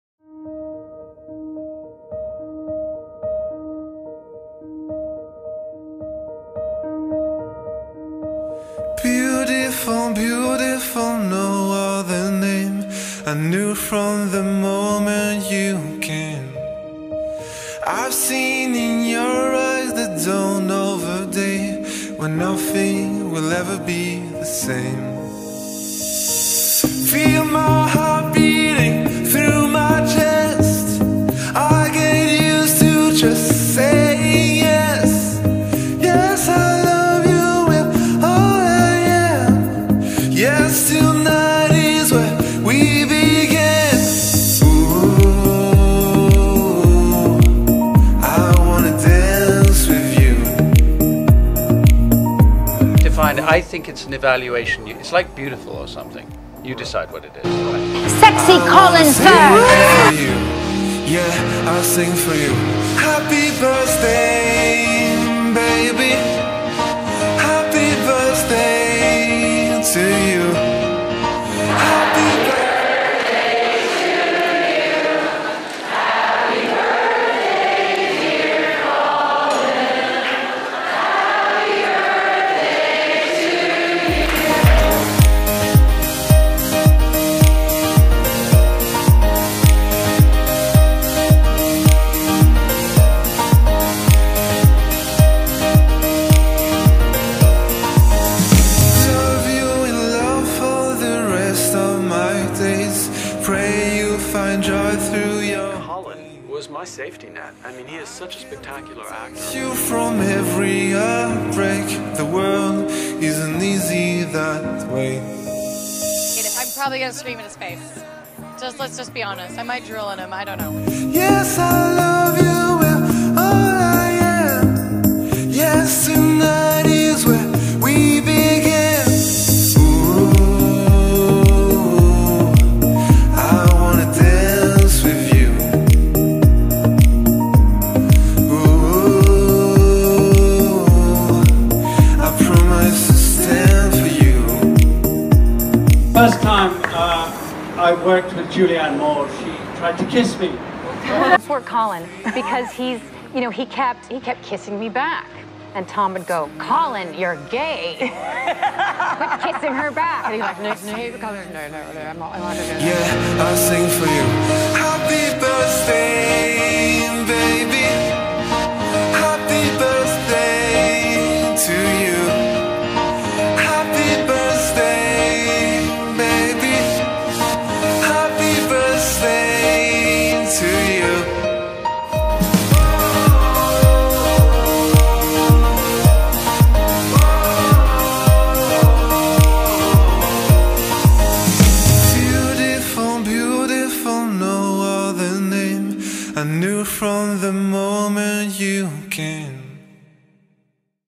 happy bday Superman! Happy birthday Colin Firth.
10 September 1960.
I  dedicate this video to my fav person in the whole world.
Happy bday to my superhero.